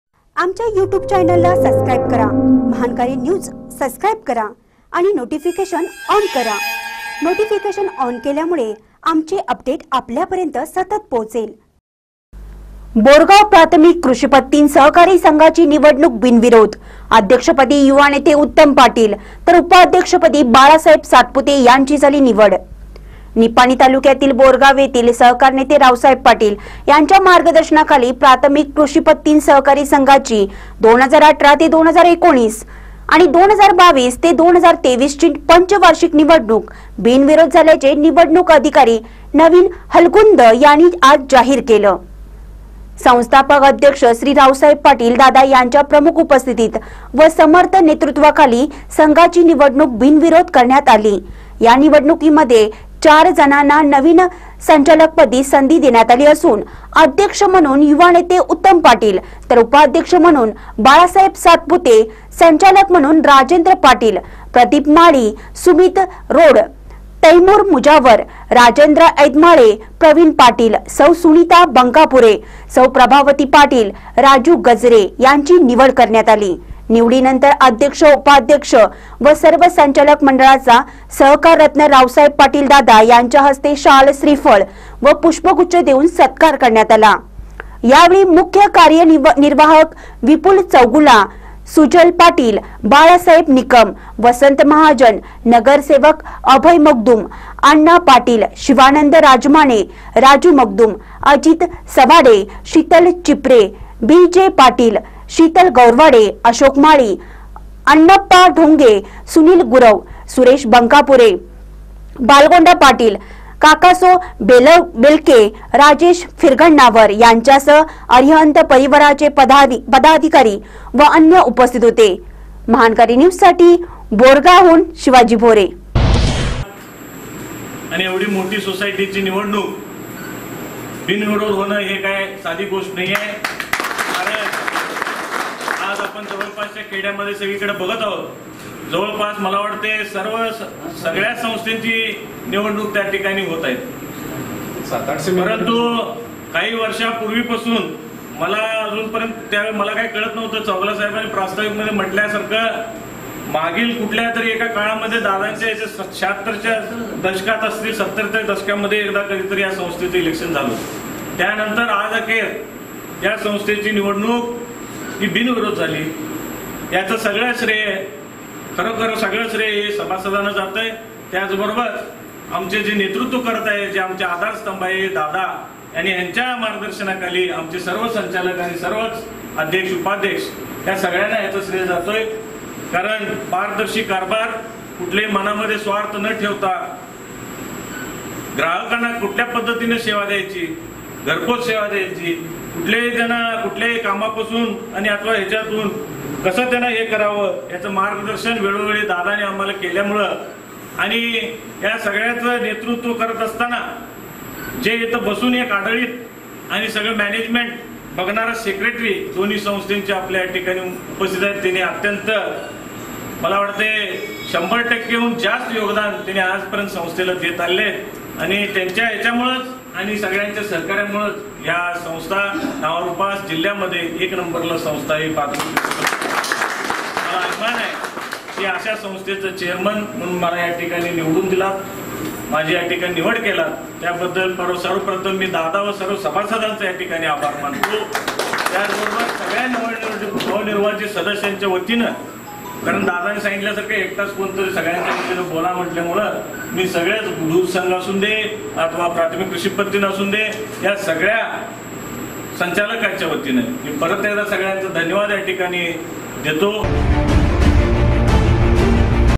बोरगाव प्राथमिक कृषी पत्तीनं सहकारी संघाची निवडणूक बिनविरोध
बोरगाव प्राथमिक कृषी पत्तीनं सहकारी संघाची निवडणूक बिनविरोध ; अध्यक्षपदी युवा नेते उत्तम पाटील तर उपाध्यक्षपदी बाळासाहेब सातपुते यांची झाली निवड